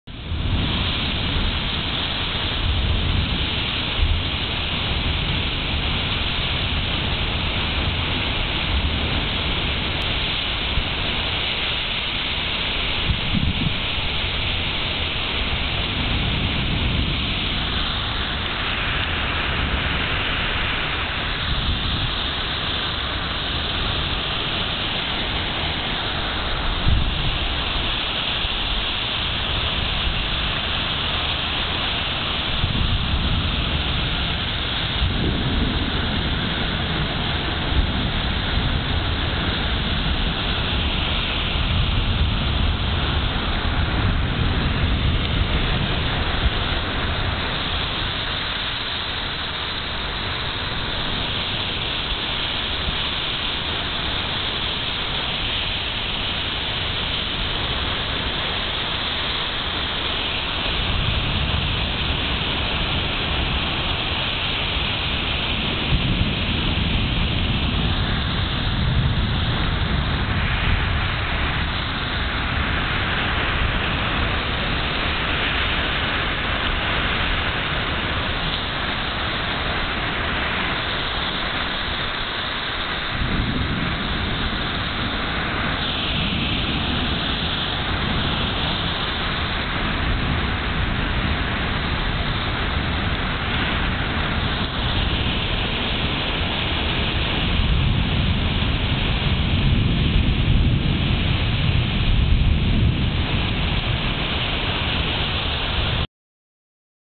raining... heavy... 3
tornado watch for Chicago and suburbs, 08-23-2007